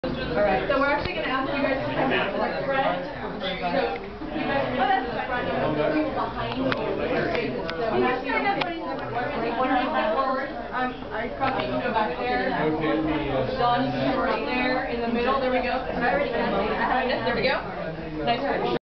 Telomolecular SCAM Ribbon cutting ceremony
Here is a video I took at Rancho Cordova Location where the office used to be back in March of 2008. Matthew Sarad is the one in the middle in the light grey suit with scissors in hand.

Hopefully this asshole will Go to prison soon!   Here is some more info about this con artist.